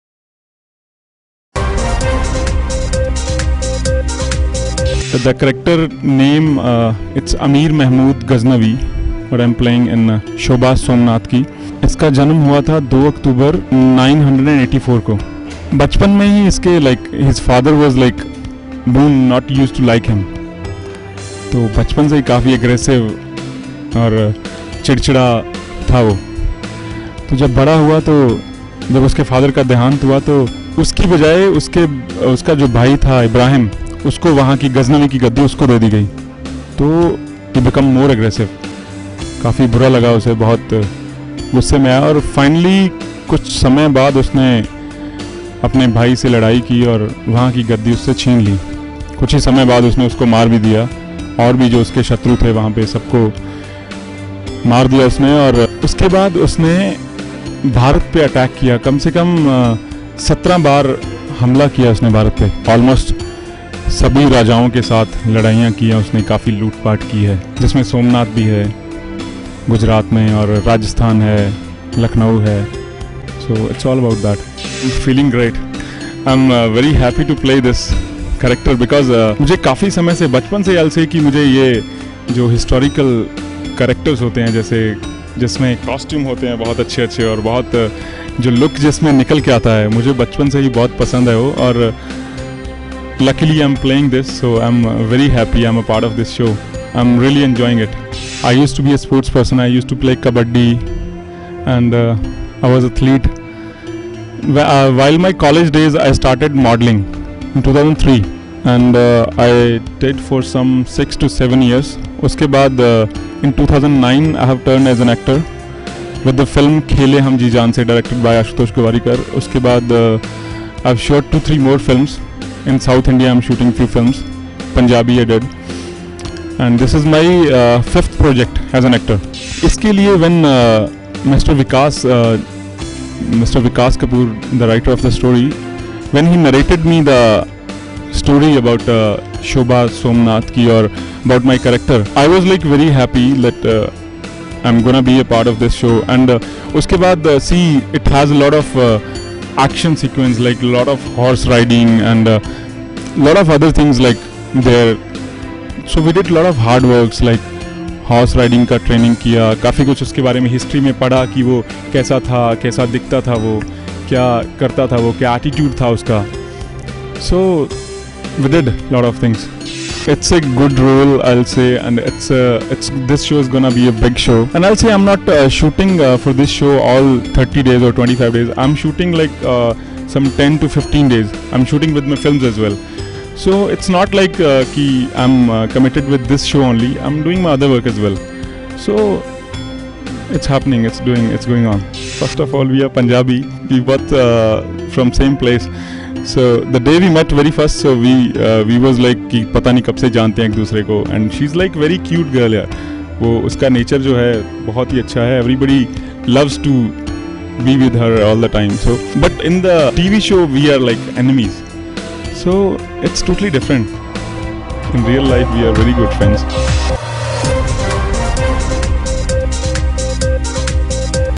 Adgully Exclusive | Vikramjeet Virk aka Mohammad Ghaznavi of Shobha Somnath Ki, unplugged!
Vikramjeet Virk, the talented actor who has bagged the role of 'Mohammad Ghaznavi' in Zee TV's show Shobha Somnath Ki. He made his big screen debut with the Ashutosh Gowarikar periodic movie, Kheley Hum Jee Jaan Se. He is also shooting for a movie down South titled Casanova with renowned actor Mohan Lal.